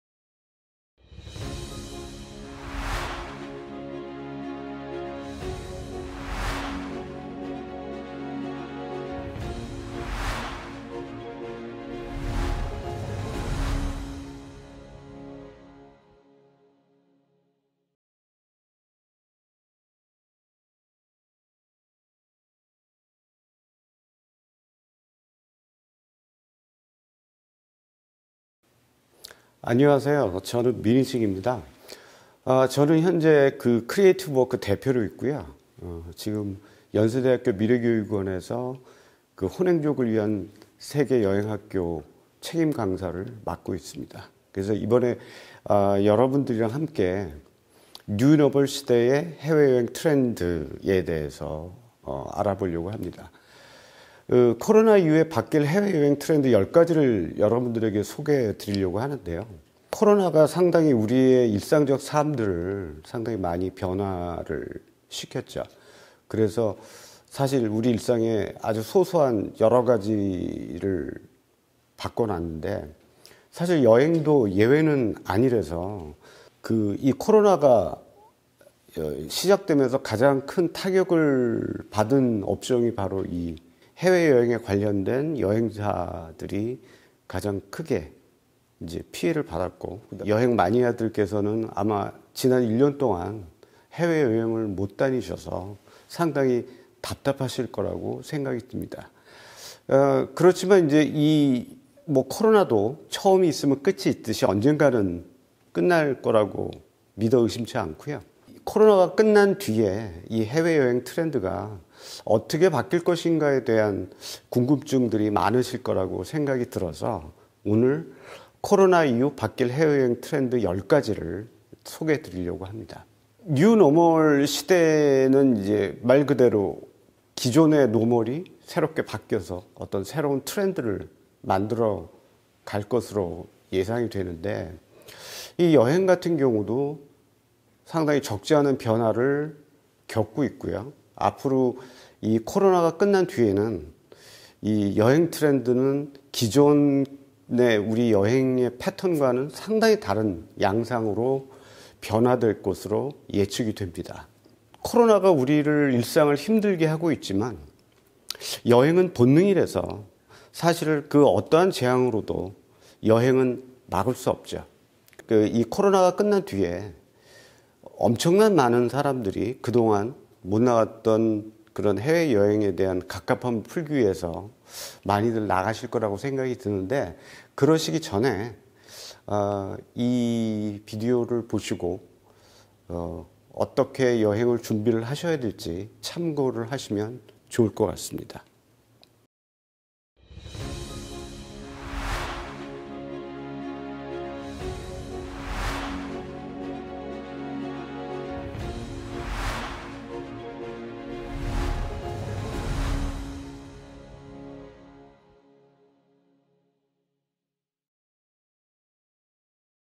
[연세대학교 미래교육원] 뉴노멀 시대의 여행 트렌드ㅣ민희식 강사ㅣ연세대학교 부설 평생교육원
연세대학교 미래교육원 공식 홈페이지